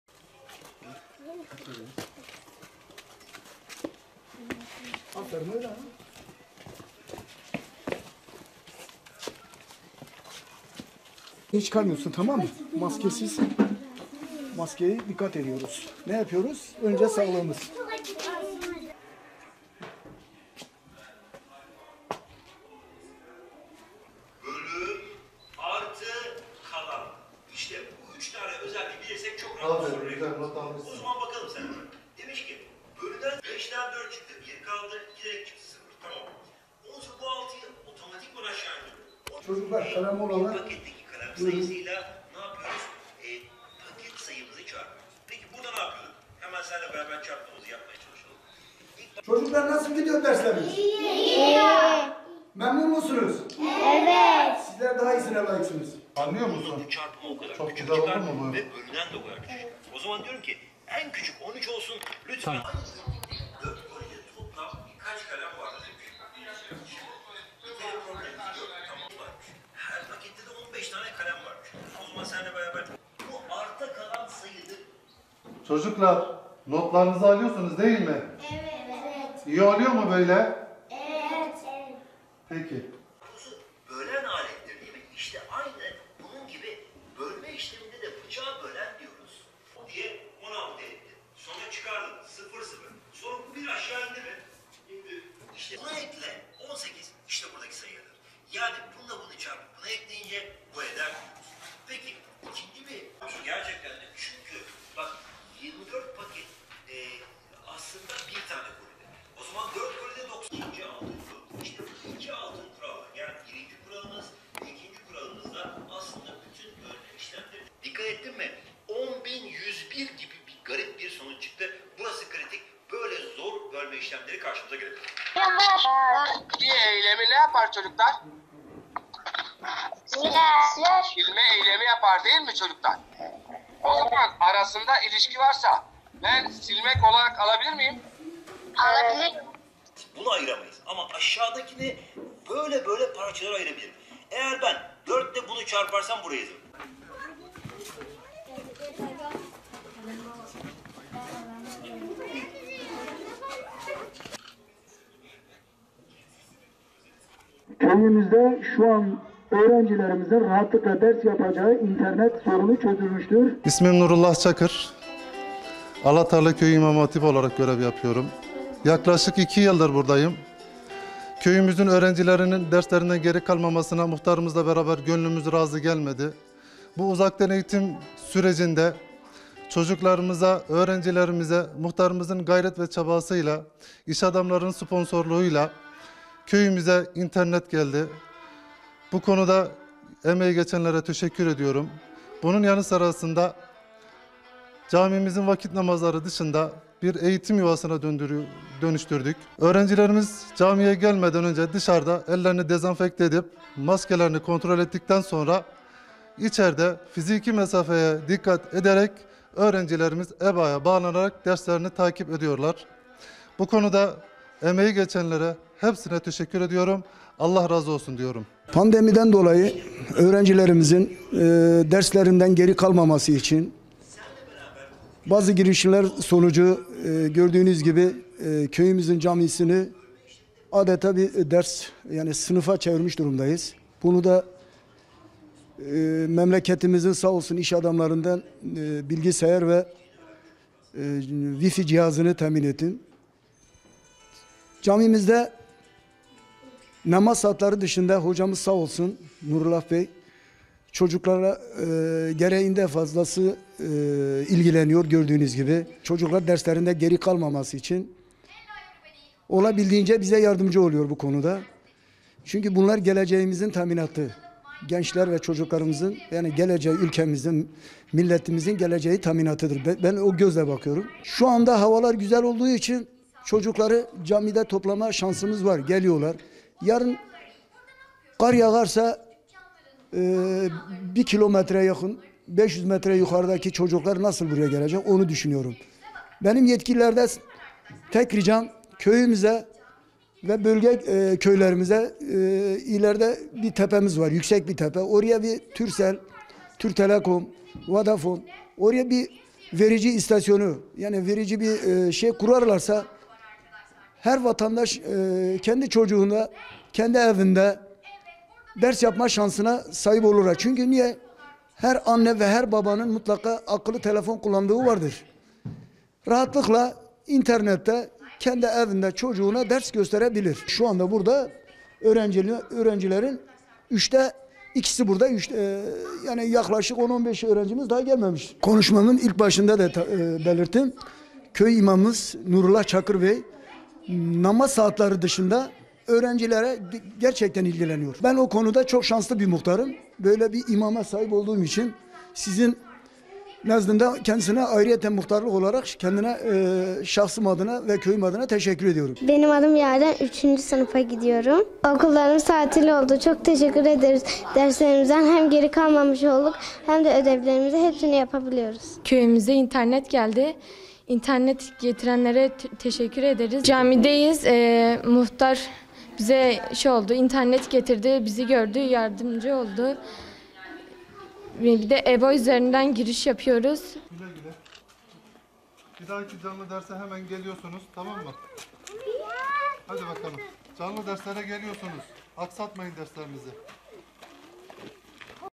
Elazığ'da cami depremzede öğrencilerin eğitim yuvası oldu
Elazığ’da depremde ilkokulun ağır hasar gördüğü bir köyde nternet de olmayınca öğrenciler için devreye muhtar girdi. Muhtar, köyün camisine internet çektirip, bilgisayar getirterek camiyi eğitim yuvasına dönüştürdü.